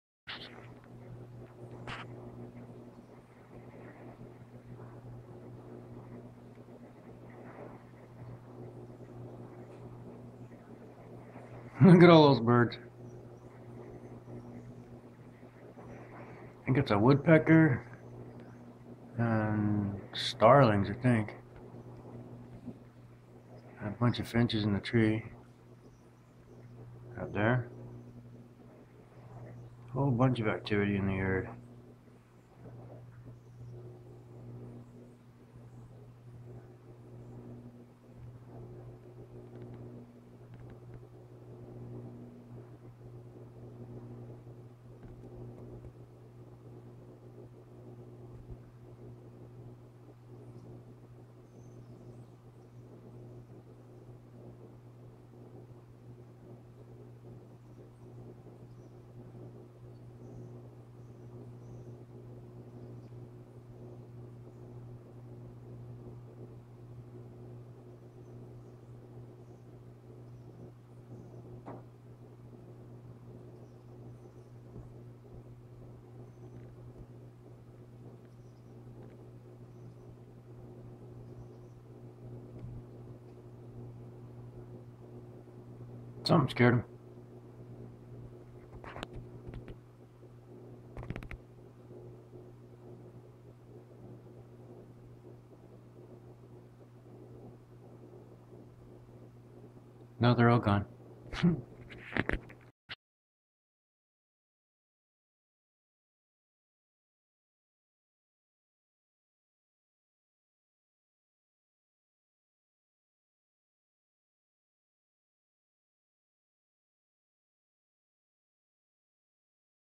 Backyard Birds
Birds around the backyard birdfeeder.  My cat likes this.
Not sure of the species.